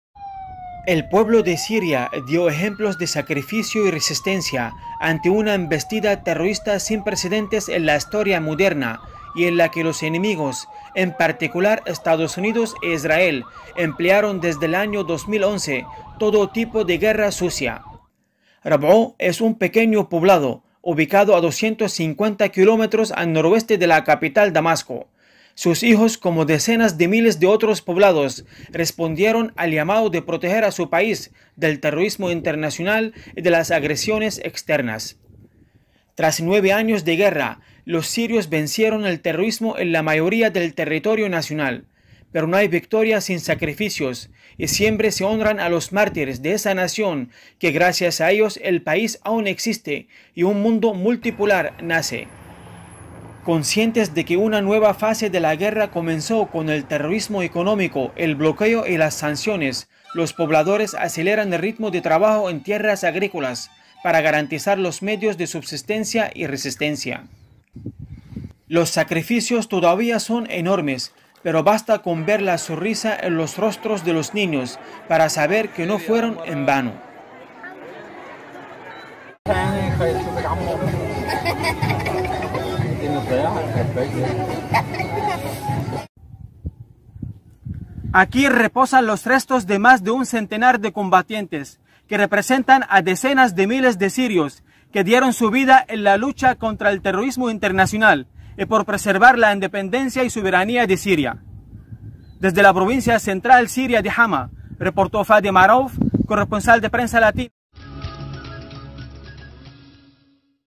Sacrifice and resistance: the Syrian people defy bestial terrorism
Sacrifice and resistance: the Syrian people defy bestial terrorism. Prensa Latina (Cuba) report on the examples of sacrifice and resistance of the Syrian people, "without precedent in modern history", in the face of bestial terrorism, directed by imperialism. Fady Marouf reports from Damascus and Hama.